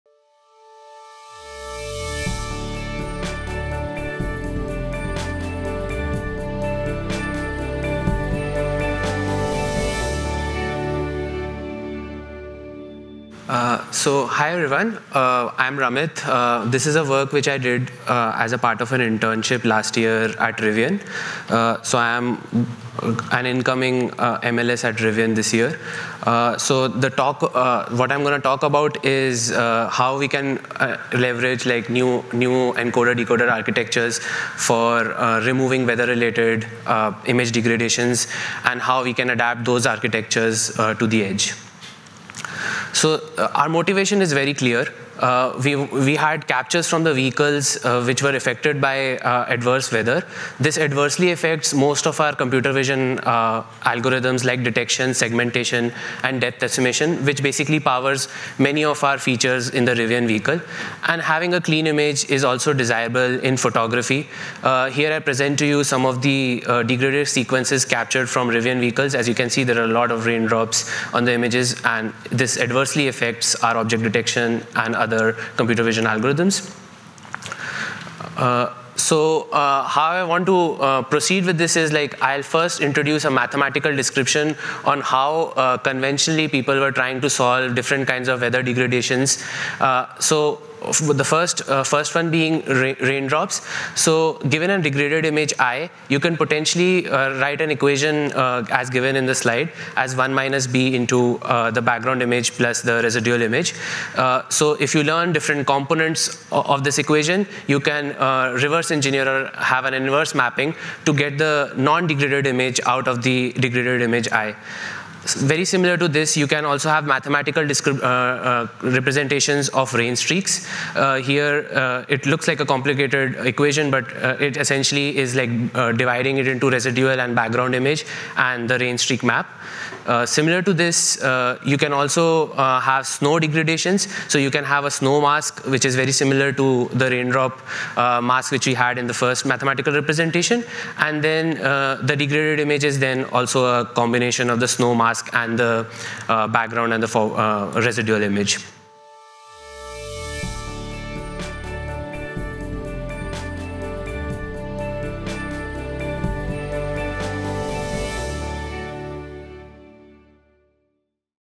Rivian Explains How to Remove Weather-related Image Degradation at the Edge (Preview)
For the full version of this video, along with hundreds of others on various edge AI and computer vision topics, please

Ramit Pahwa, Machine Learning Scientist at Rivian, presents the “Removing Weather-related Image Degradation at the Edge” tutorial at the May 2024 Embedded Vision Summit.

For machines that operate outdoors—such as autonomous cars and trucks—image quality degradation due to weather conditions presents a significant challenge. For example, snow, rainfall and raindrops on optical surfaces can wreak havoc on machine perception algorithms. In this talk, Pahwa explains the key challenges in restoring images degraded by weather, such as lack of annotated datasets, and the need for multiple models to address different types of image degradation.

Pahwa also introduces metrics for assessing image degradation. He then explains Rivian's solutions and shares results, demonstrating the efficacy of transformer-based models and of a novel, language-driven, all-in-one model for image restoration. Finally, he highlights the techniques used to create efficient implementations of Rivian's models for deployment at the edge—including quantization and pruning—and shares lessons learned from implementing these models on a target processor.